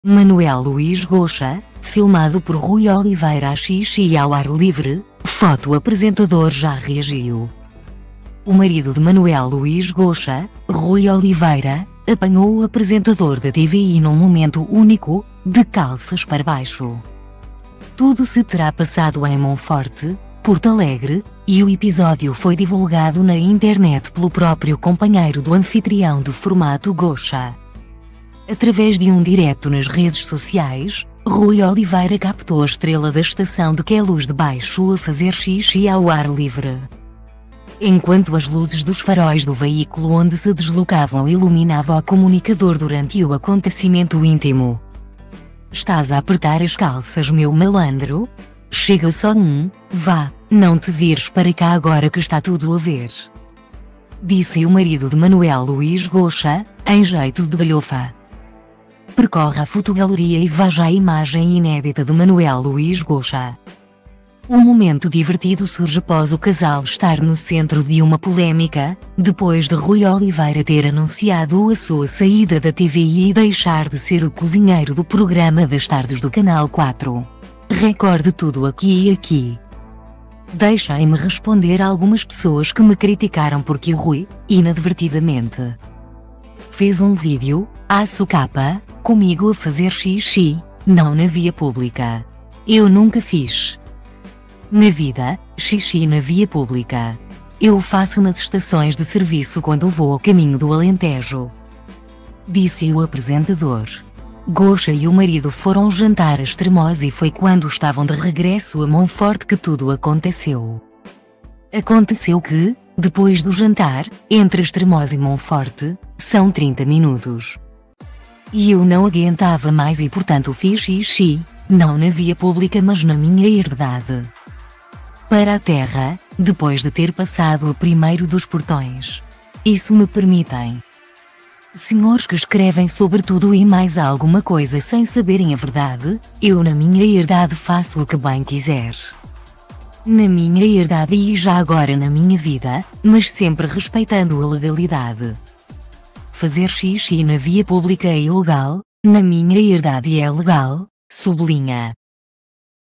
Manuel Luís Goucha - Filmado por Rui Oliveira a xixi ao ar livre (foto).Apresentador já reagiu
O marido de Manuel Luís Goucha, Rui Oliveira, ‘apanhou’ o apresentador da TVI num momento único: de calças para baixo.Tudo se terá passado em Monforte, Portalegre, e o episódio foi divulgado na Internet pelo próprio companheiro do anfitrião do formato “Goucha”.